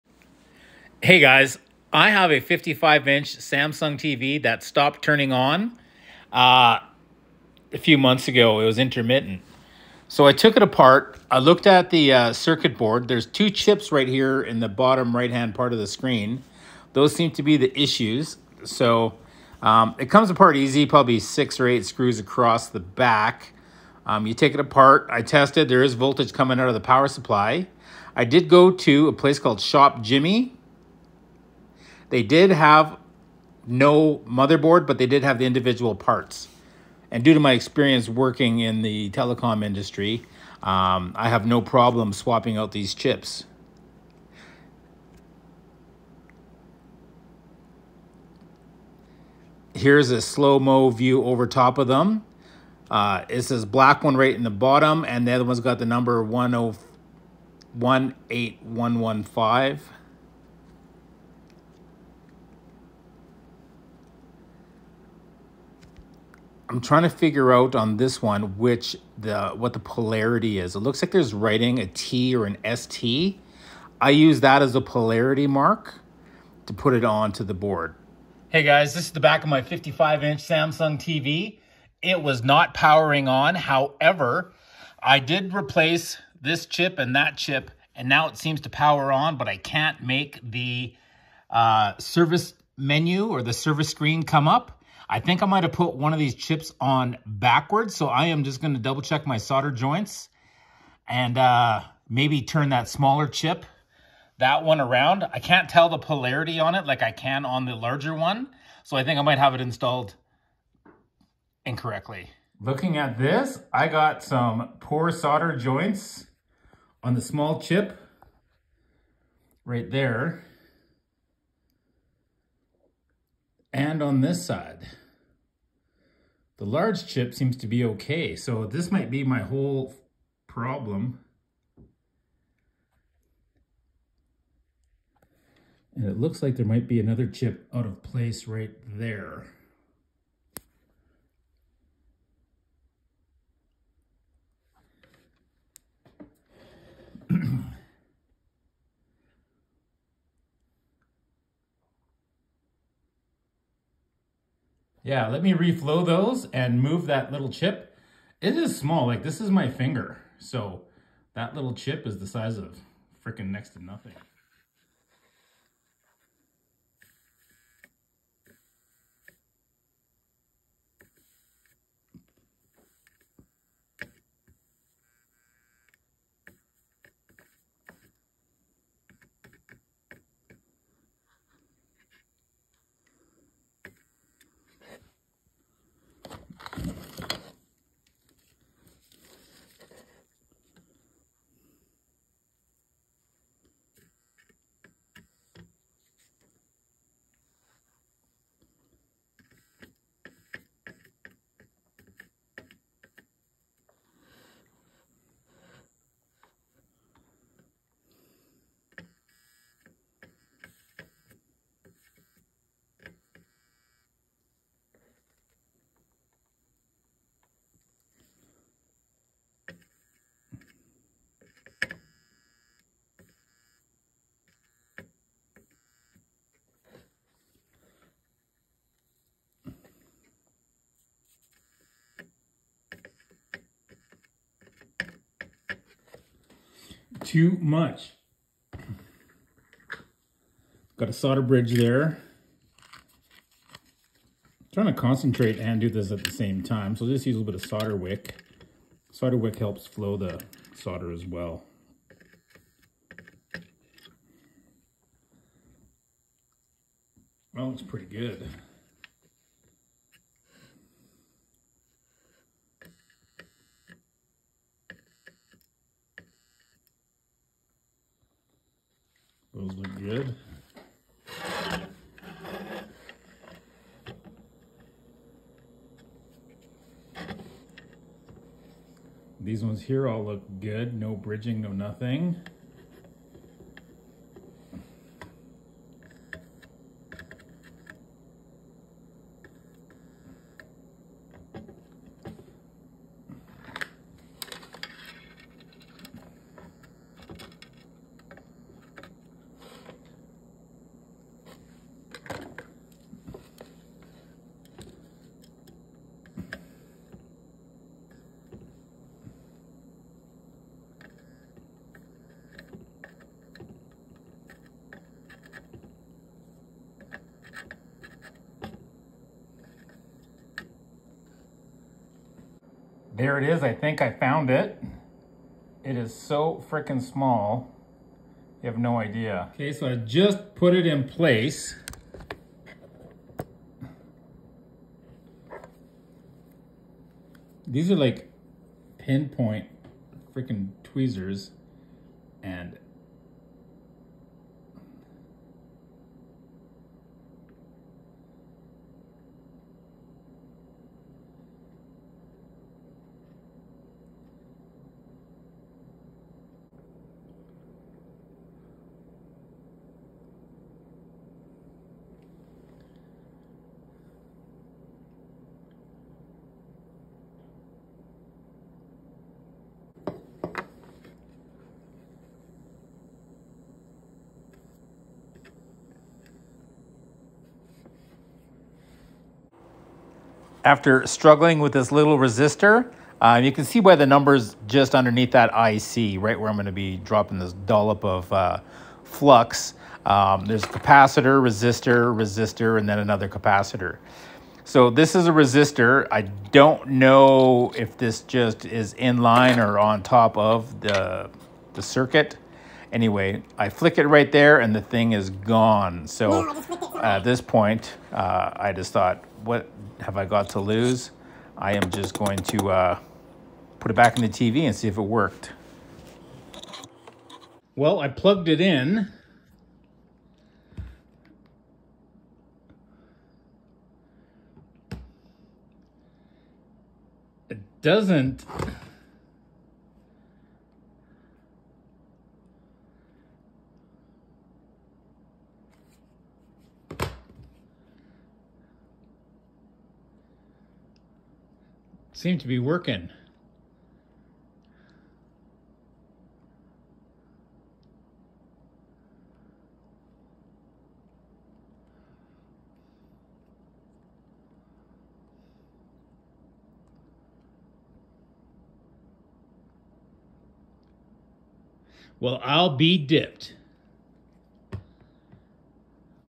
Samsung 55inch not turning on. 55EH600
55 inch Samsung Tv not turning, replacement board not available but ShopJimmy had the parts in stock.  Very tiny parts and I have experience with Printed Circuit boards so this was pretty easy for me.   But since the TV was a brick no harm in trying was my thoughts.
These TV have about 8 parts, 3 Mother boards, do depending on what yours is doing or not doing, youtube has video and probably a solution to your problem.